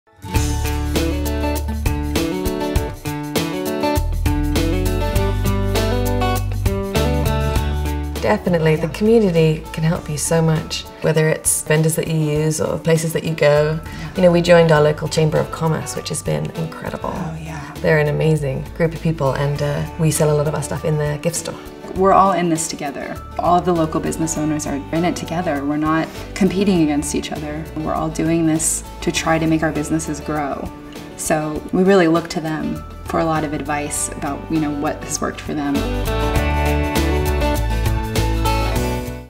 Getting Advice, Episode 1
Finding good advice is just a matter of knowing where to look. In this episode, Moonlight Makers share a few places to find business advice.